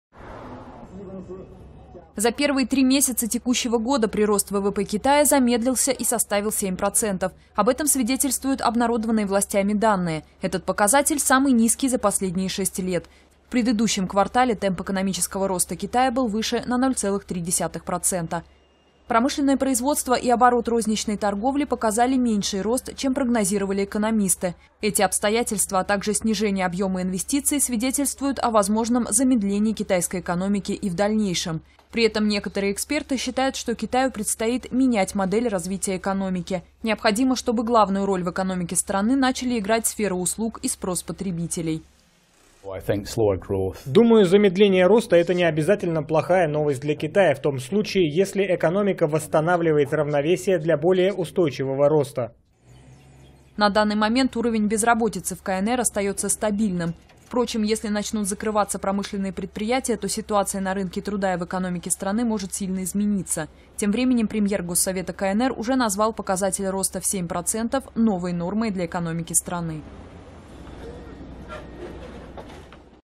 Китайская экономика замедляет рост (новости)
Китайская экономика замедляет рост. За первые три месяца текущего года прирост ВВП Китая замедлился и составил 7%. Об этом свидетельствуют обнародованные властями данные. Этот показатель – самый низкий за последние шесть лет. В предыдущем квартале темп экономического роста Китая был выше на 0,3%.

Промышленное производство и оборот розничной торговли показали меньший рост, чем прогнозировали экономисты. Эти обстоятельства, а также снижение объёма инвестиций свидетельствуют о возможном замедлении китайской экономики и в дальнейшем.

При этом некоторые эксперты считают, что Китаю предстоит менять модель развития экономики. Необходимо, чтобы главную роль в экономике страны начали играть сфера услуг и спрос потребителей.

[Эндрю Колкухоун, аналитик Fitch]:
«Думаю, замедление роста – это необязательно плохая новость для Китая в том случае, если экономика восстанавливает равновесие для более устойчивого роста».

На данный момент уровень безработицы в КНР остается стабильным. Впрочем, если начнут закрываться промышленные предприятия, то ситуация на рынке труда и в экономике страны может сильно измениться.

Тем временем премьер Госсовета КНР уже назвал показатель роста в 7% «новой нормой» для экономики страны.